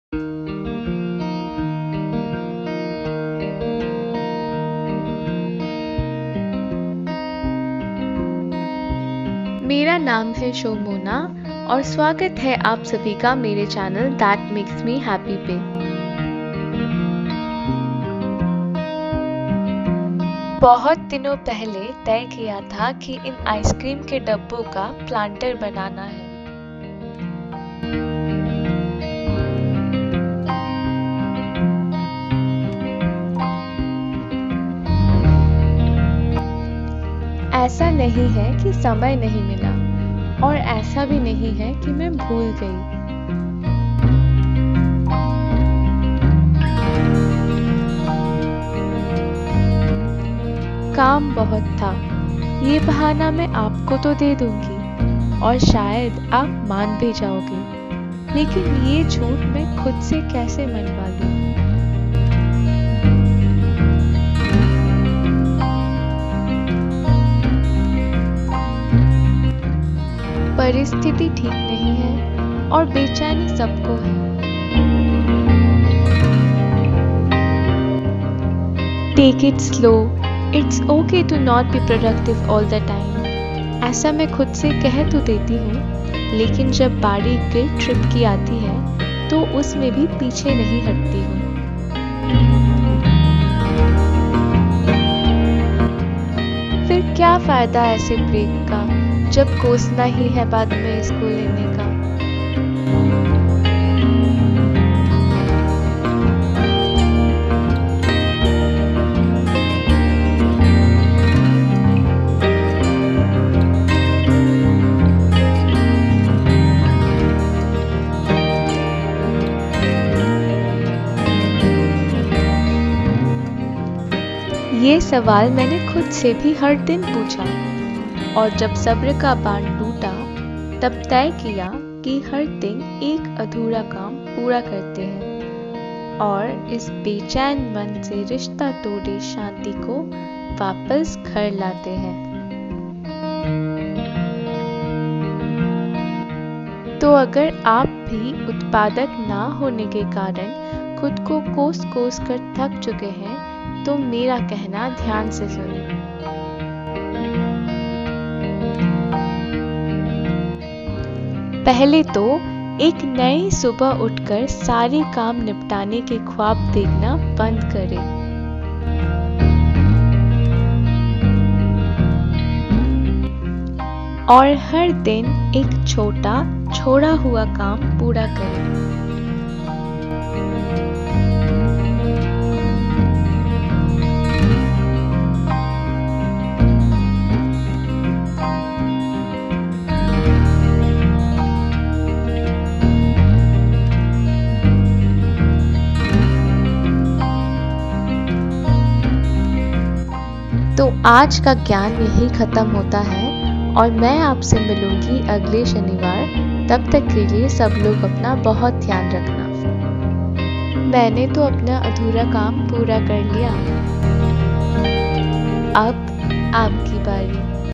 How to stop procrastinating (Hindi) | DIY Planter from ice-cream tub
This is an aesthetic video where I diy a planter and and talk about how to stop procrastinating in Hindi.
I have tried to make this an aesthetic video. Hope this motivates you guys to stop procrastinating and start doing. Let us conquer every resolution we make this year, start fresh and be our best selves.

Saumona

Copyright:-
Everything you see on this video was created by Saumona Basu unless otherwise stated.